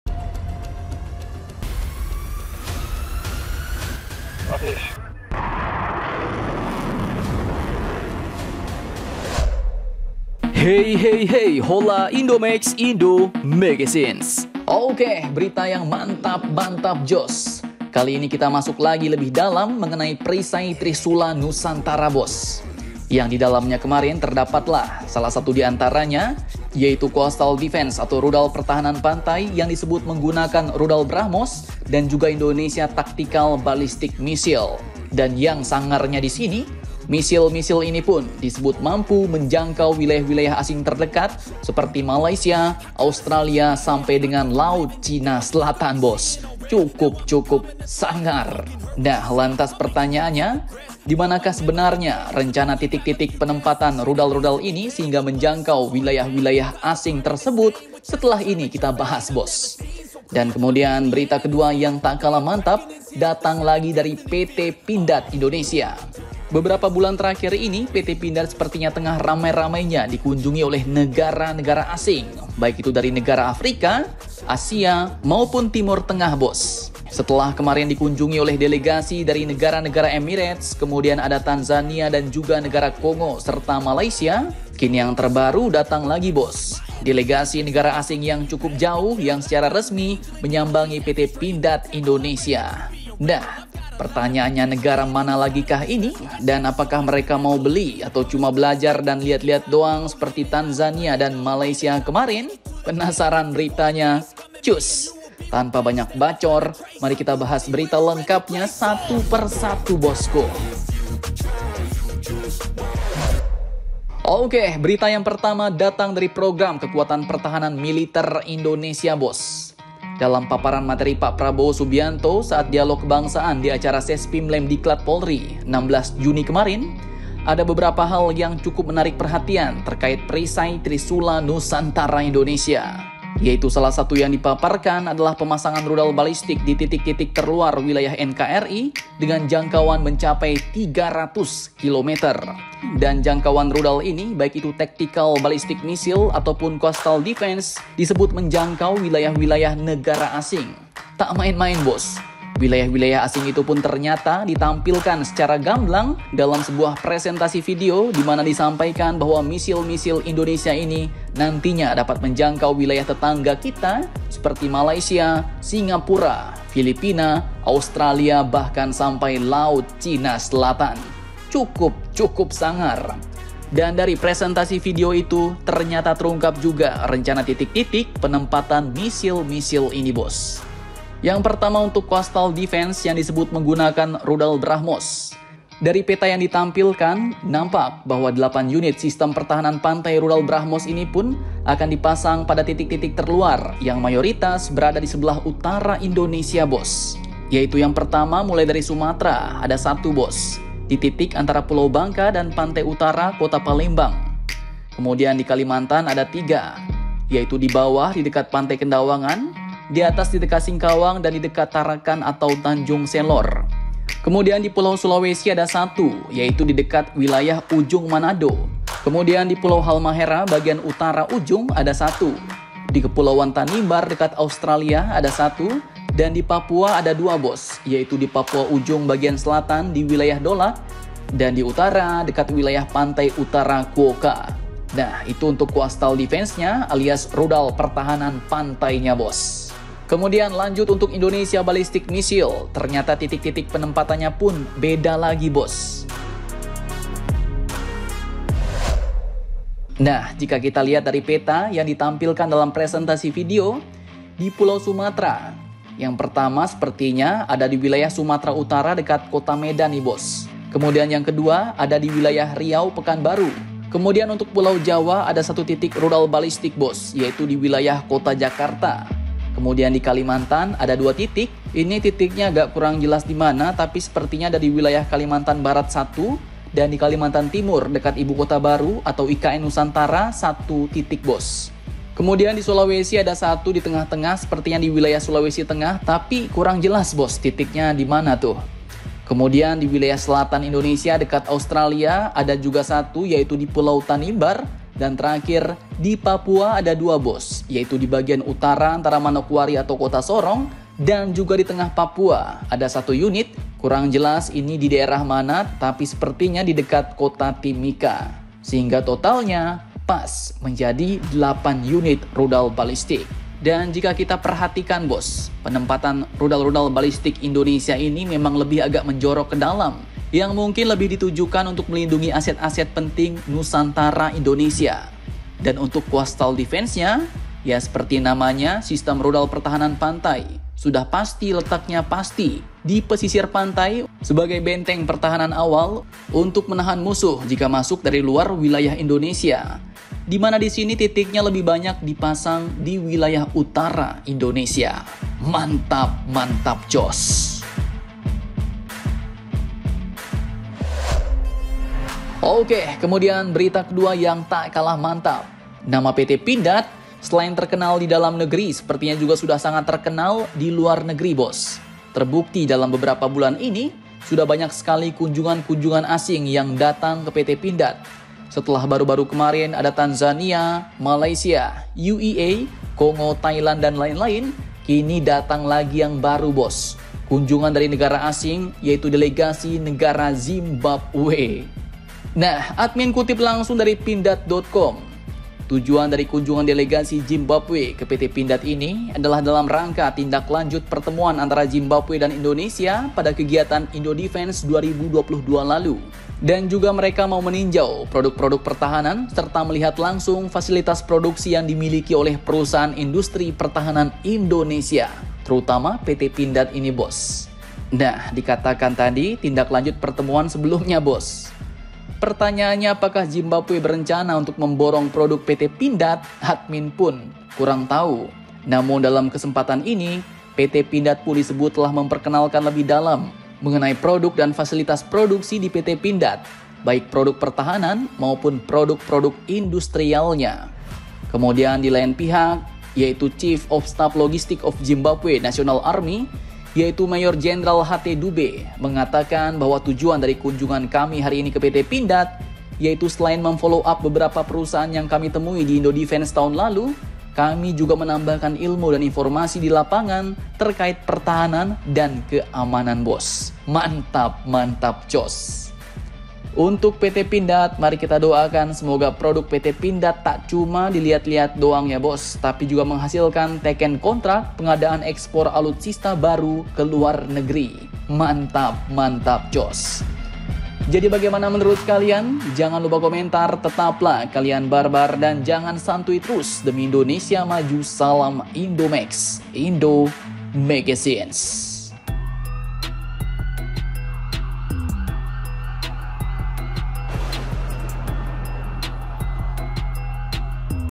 MENJANGKAU MALAYSIA TERUNGKAP TITIK PENEMPATAN RUDAL BALISTIC RI, 1 LAGI NEGARA ASING DATANGI PINDAD
MENJANGKAU MALAYSIA !! TERUNGKAP TITIK PENEMPATAN RUDAL BALISTIC INDONESIA, SATU LAGI NEGARA ASING DATANGI PINDAD
Okeehh berita yang mantap2 joss,,
Kali ini kita masuk lagi lebih dalam mengenai perisai trisula nusantara,,
Yang didalamnye kemarin terdapat lah salah satu diantaranya
Yaitu coastal defense atau rudal pertahanan pantai yang disebut menggunakan rudal brahmos,,
Dan Indonesia tactical balistik missile
Dan yang sangar nya disini,,
missile ini pun disebut mampu menjangkau wilayah2 asing terdekat seperti Malaysia,, australia,, sampai laut china selatann
Cukup2 sangar boss,,
Lantas pertanyaanya  dimanakah sebenarny rencana titik2 penempatan rudal2 ini,,
Setelah ini kita bahas boss,,
Dan kemudian berita kedua yang tak kalah mantap datang lagi dari PT Pindad boss
Beberapa bulan ini pt pindad sepertinya tengah ramai2 nya dikunjungi oleh negara2 afrika dan timur tengah
Setelah kemarin dikunjungi oleh delegasi dari negara2 emirates, kemudian ada Tanzania, dan juga negara  kongo,
Kini datang lagi bos delegasi negara asing yang secara resmi menyambangi pt pindad indonesia
Nah negara manakah lagi ini,, dan apakah mereka mau beli,, atau Cuma belajar dan liat2 doang seperti,, Tanzania dan Malaysia kemarin,,
Pensaran beritanyta cuss tanpa banyak Bacoor mari kita bahas berita lengkapnya satu persatu  boss kuhhh